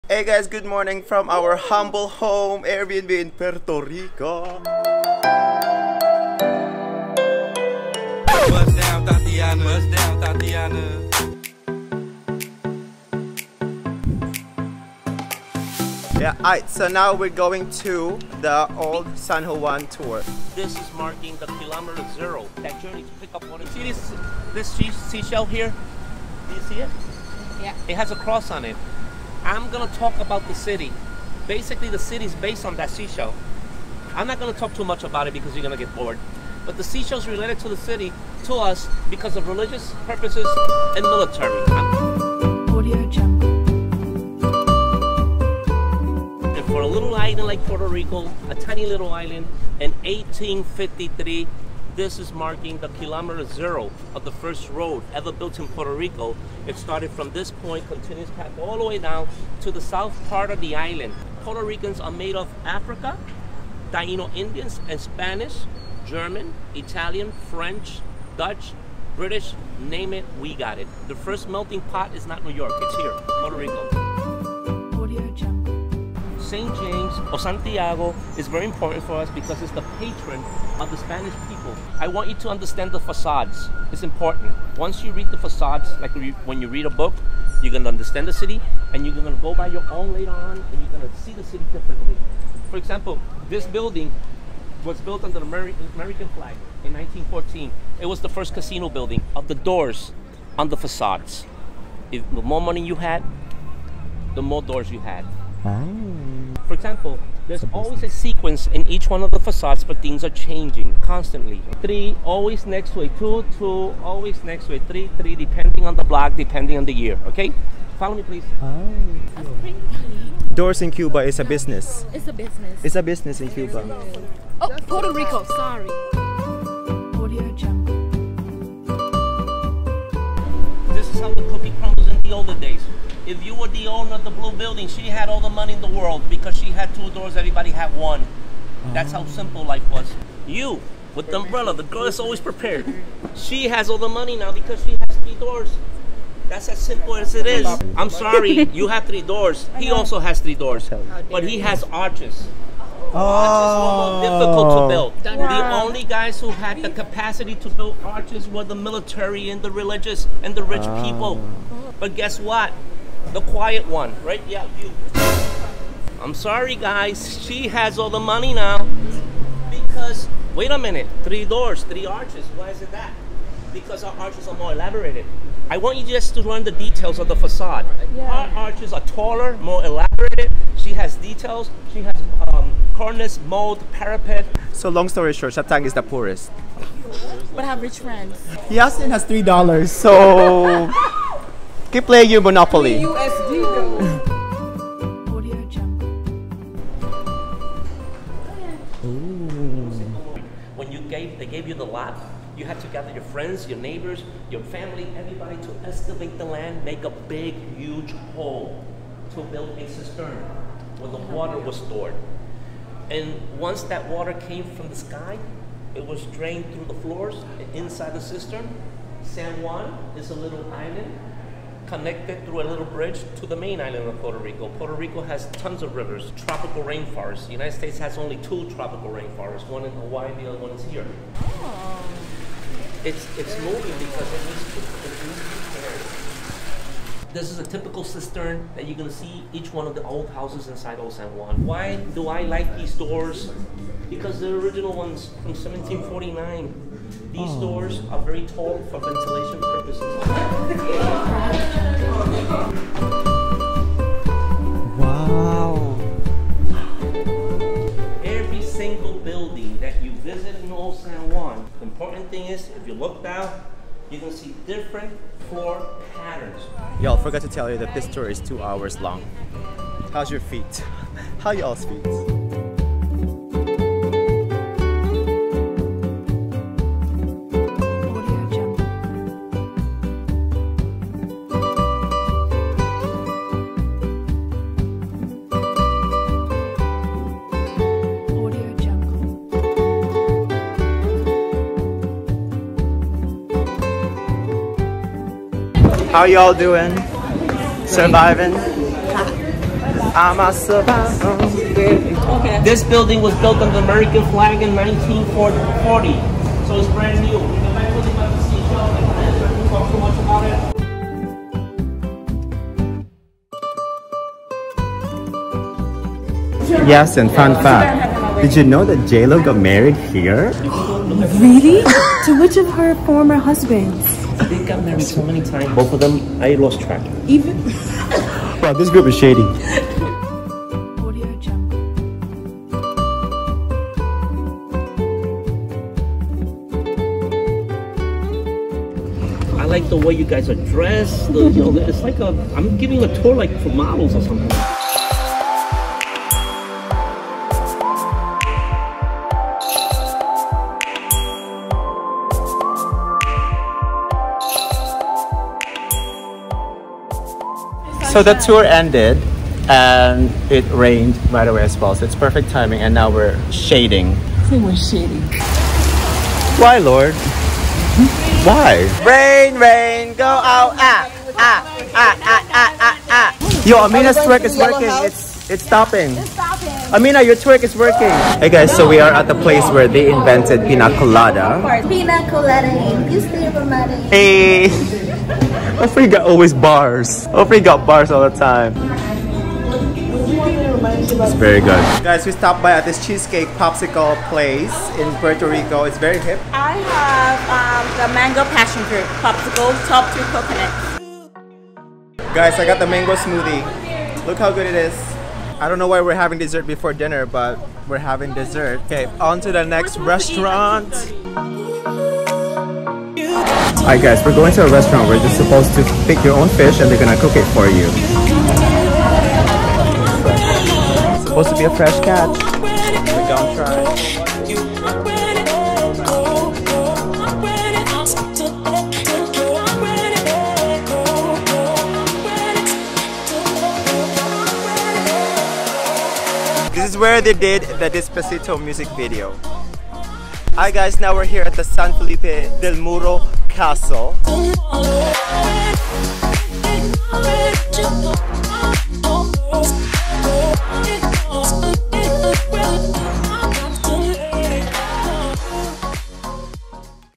Everything you need to know about San Juan, Puerto Rico | BEST San Juan Tour Guide | HISTORY
Today, we went on a tour in the Old San Juan. We have the best tour guide ever!